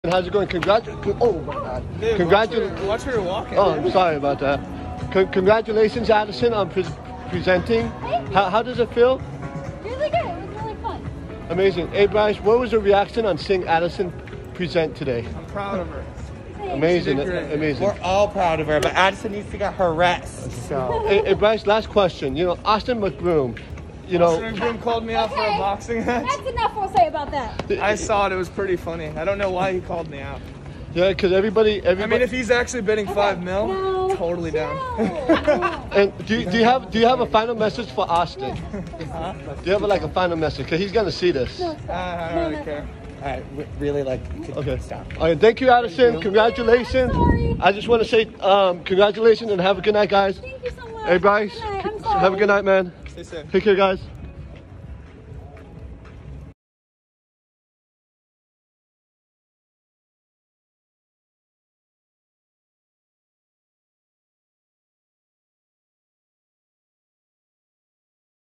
Addison Rae Calms Bryce Hall Down As He Speaks On Austin McBroom $5,000,000 Bet & Says It's On !!!
AddisonRae Calms BryceHall Down As He Speaks On AustinMcBroom $5,000,000 Bet & Says It's On !!!